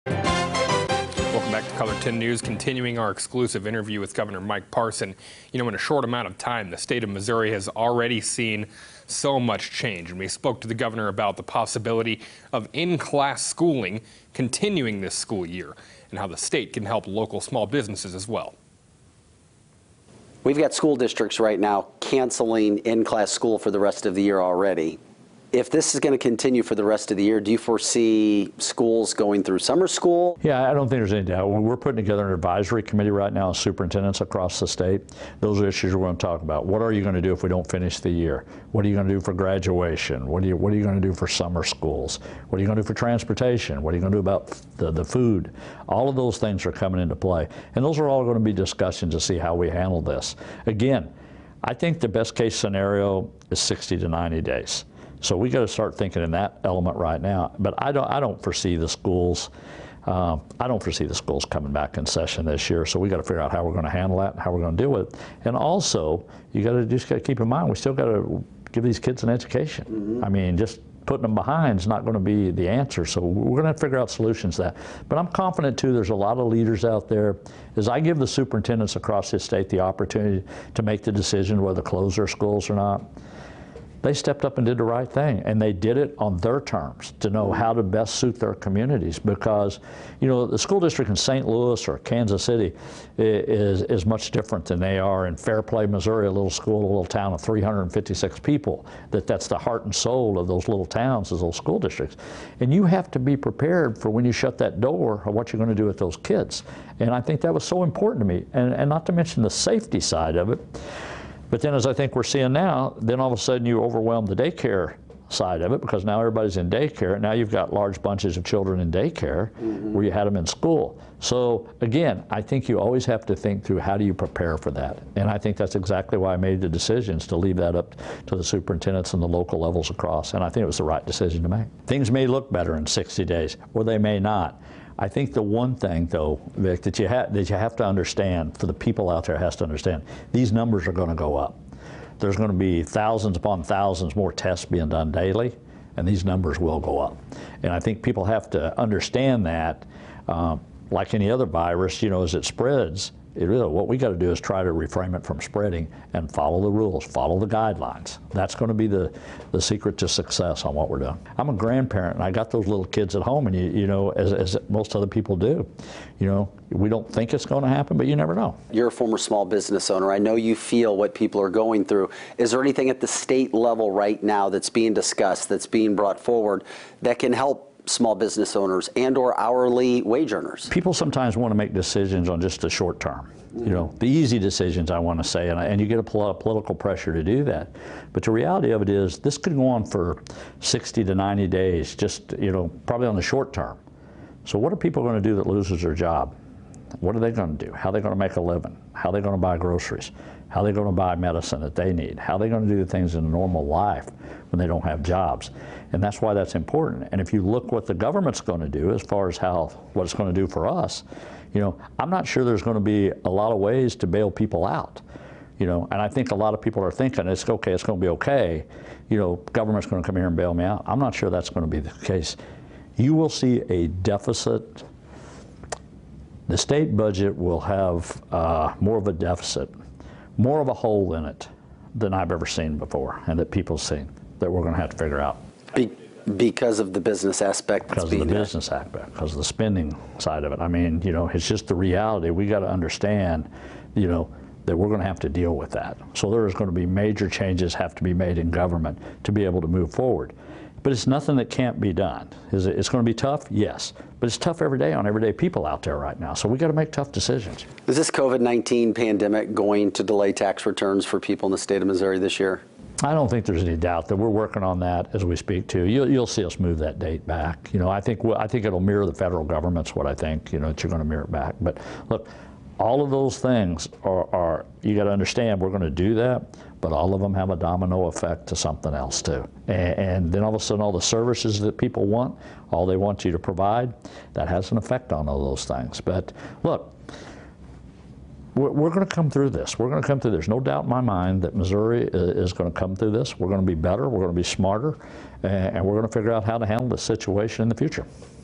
EXCLUSIVE INTERVIEW: PART 4
JEFFERSON CITY, Mo. -- We're bringing you an exclusive interview with Missouri Gov. Mike Parson.

We talk about the opportunity to use local technology in helping fight COVID-19 and virus' in the future, when we will get more testing, and his plan now that a state representative was infected with the virus.

INSERT HERE

 In a short amount of time the state of Missouri has already seen a lot of change.

We spoke to the governor about the possibilty of in-class schooling continuing this school year and how the state can help local small businesses.